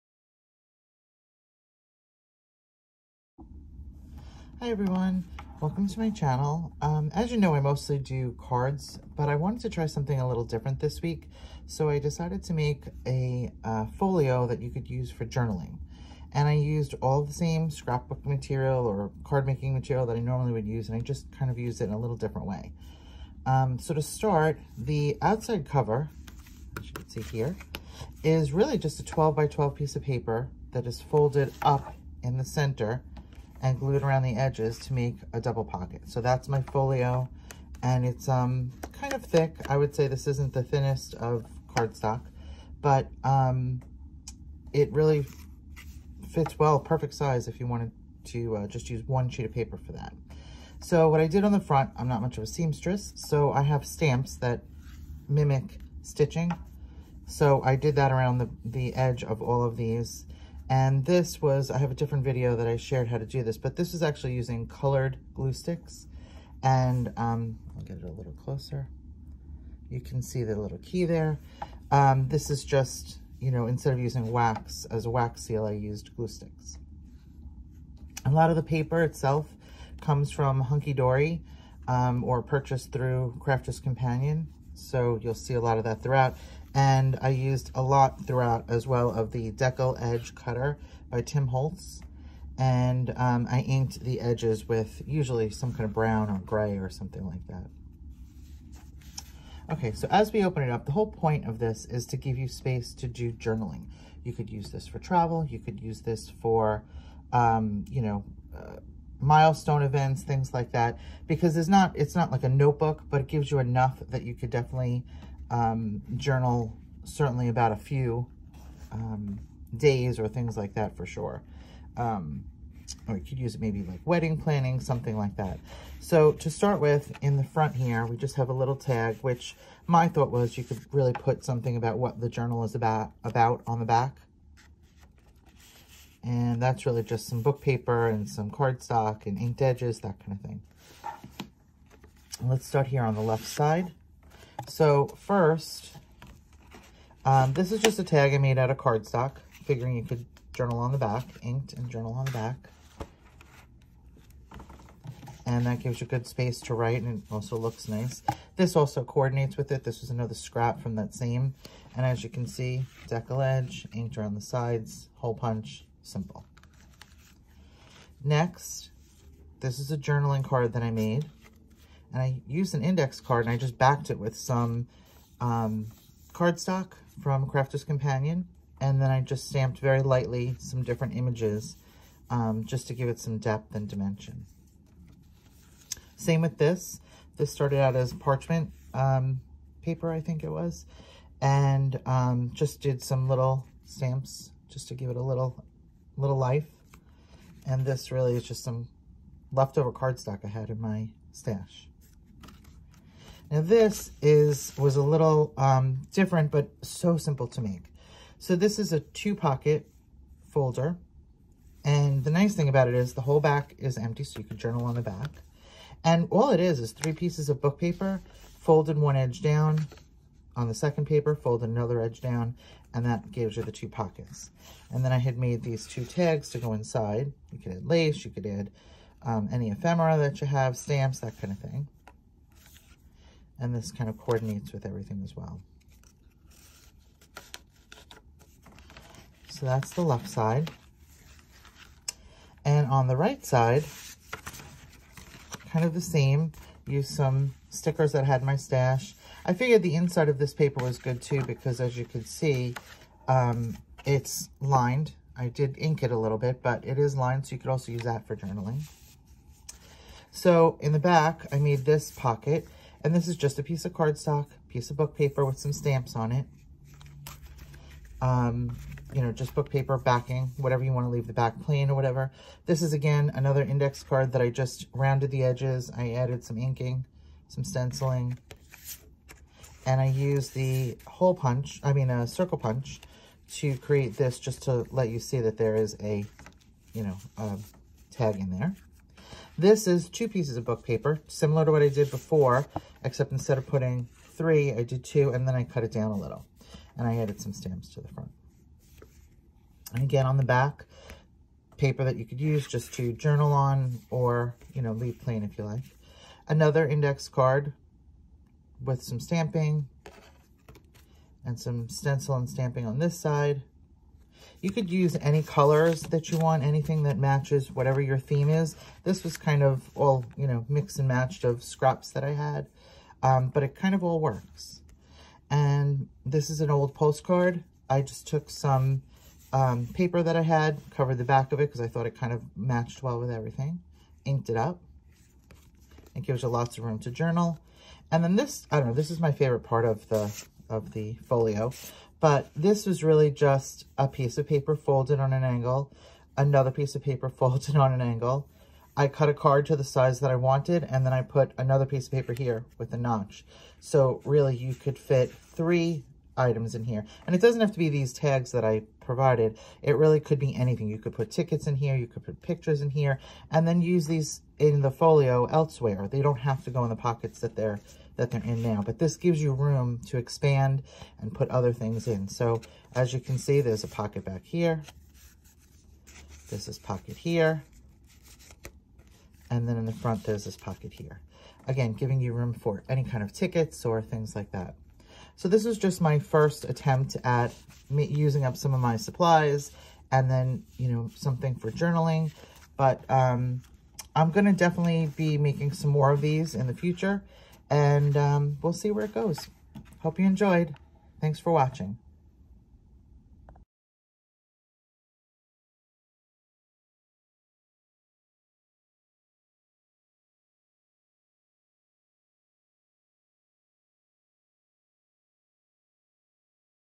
Flip Through of my first Journal Folio
Welcome back! This is my first attempt at making a journaling folio - I hope you like it!  This is a quick flip through of my folio that can be used to journal an event, milestone, vacation, whatever you want.  I have a few pockets, tags and journaling cards inside.

As for products used:

Tim Holtz deckle edge paper trimmer

Most of the cardstock and scraps are from HunkyDory Crafts and Crafter's Companion

For ink, I mainly used Distress Oxides - Tea Dye, Walnut Stain, Vintage Photo, Rusty Hinge, and Ground Espresso.

I used a variety of stamps all throughout.  Most of them were background stamps from Crafter's Companion (link above) and the stitching stamps from the front of the folio were from amazon.